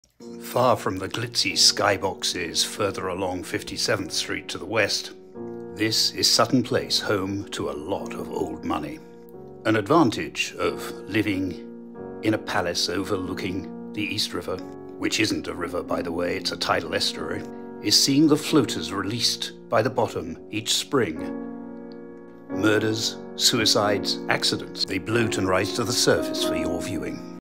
Spring Harvest New York
The East River in New York.
« It’s not a river », says Clive Burrow, « it’s a tidal estuary ».
When people are thrown in, jump in or fall in, their lungs fill with water and they sink to the bottom.  As the body’s bacteria begins to feast, gas is produced, the body swells and floats to the surface. But in winter, the ice and cold keep them down, sometimes until spring when the riverbed releases them and they bob up- or downstream (depending upon the tide).
Ah, spring in New York…